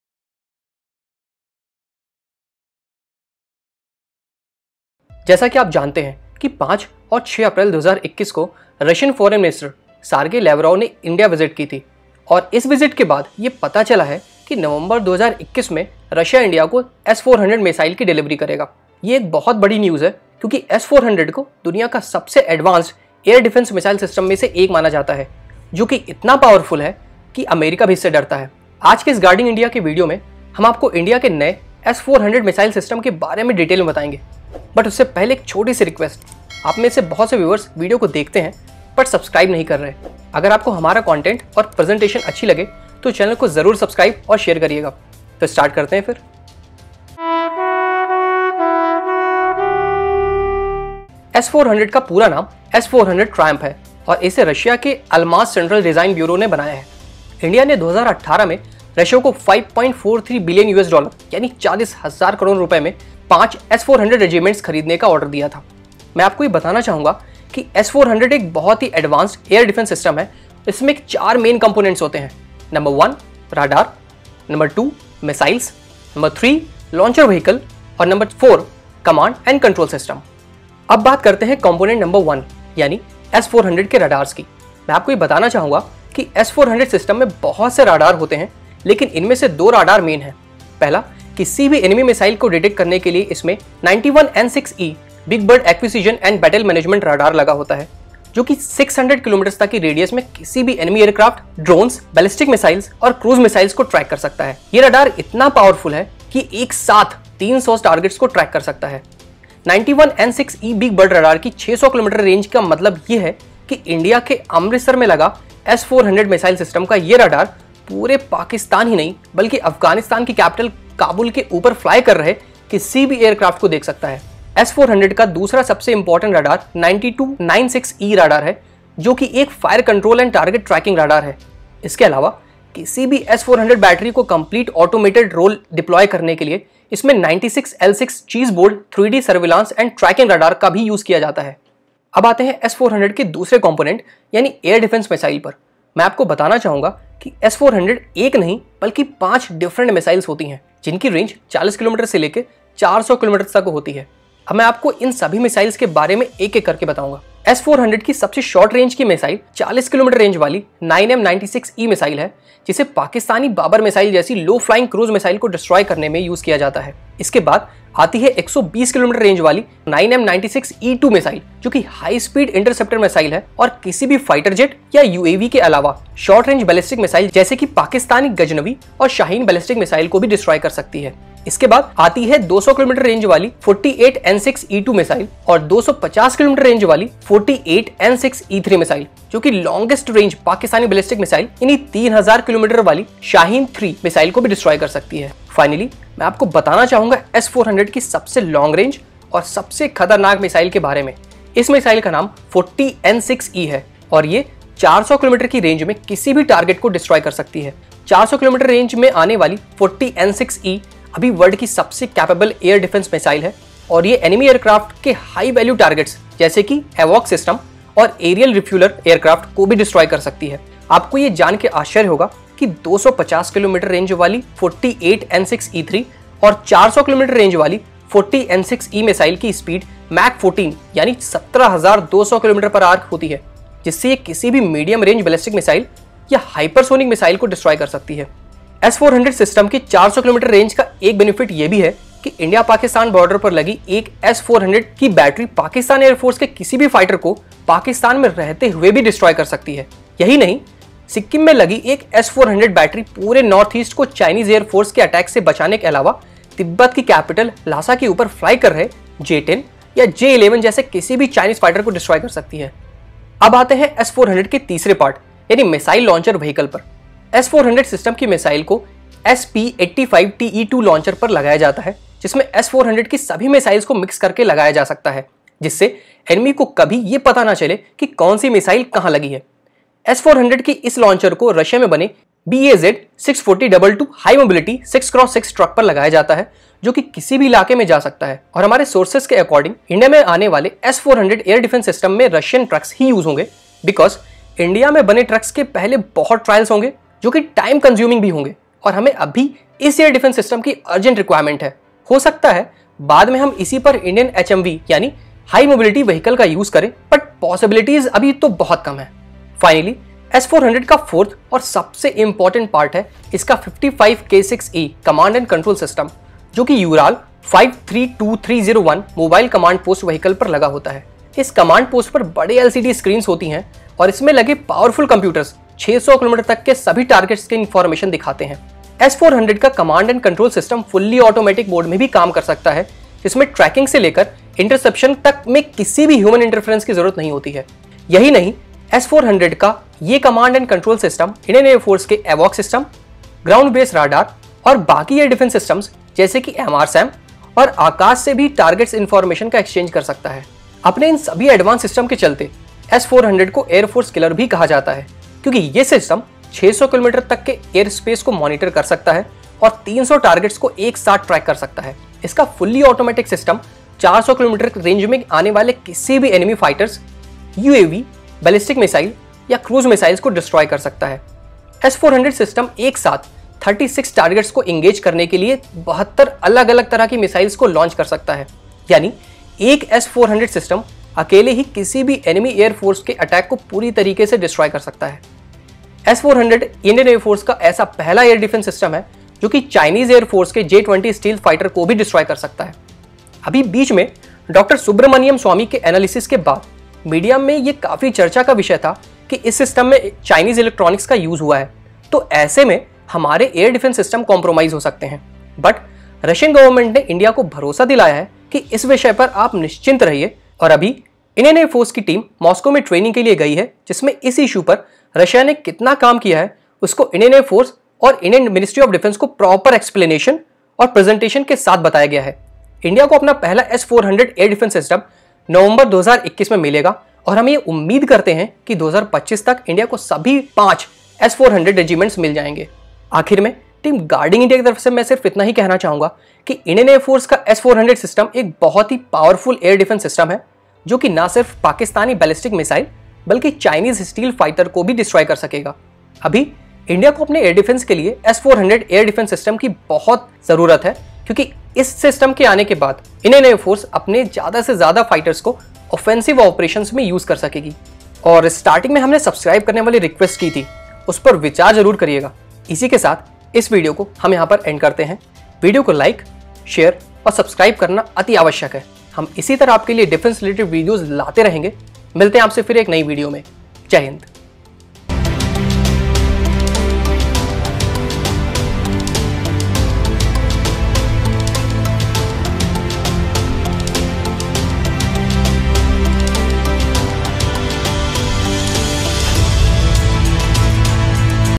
Know About India's Latest S-400 Air Defence Missile System & Dr Subramaniyam Swamy's Controversy
As you know that on 05 and 06 April 2021, Russian Foreign Minister Sergey Lavrov visited India. After the visit, it was disclosed that in November 2021 Russia will be delivering the initial batch of S-400 missiles to India.  This is a big development because S-400 is considered one of the most advanced air defence missile systems in the world. S-400 is so powerful that even America fears it. In today’s Guarding India video, we will tell you all the details about India’s new S-400 Air Defence Missile System and Dr Subramaniyam Swamy's controversy.